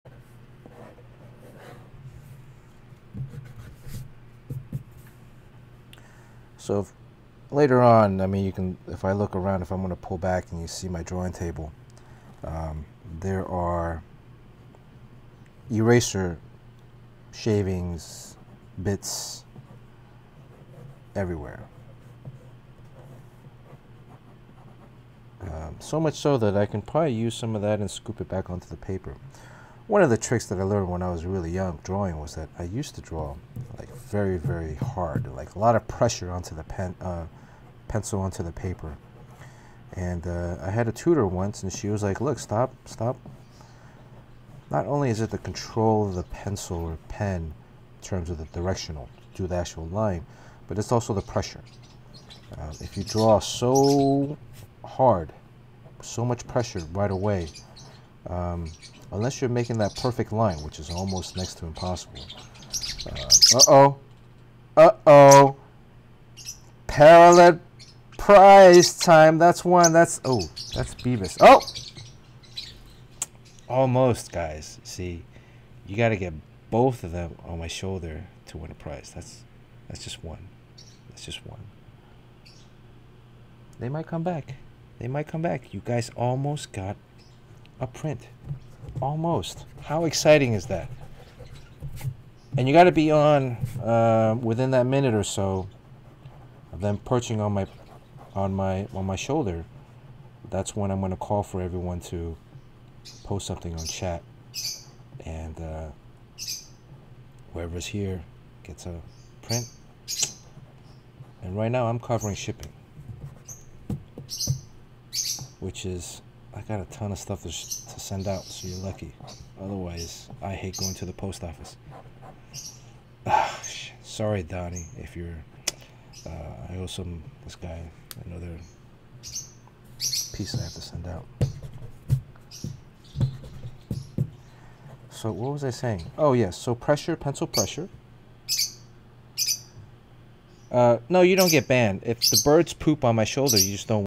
Highlight: PSR05: PENCIL PRESSURE AND (ALMOST) PARROTLET PRIZE
bernard talks erasing, pencil pressure and we almost get a PARROTLET PRIZE moment. in the PSR05 episode bernard does a single sketch of crush, one of the new members of TEEN TITANS.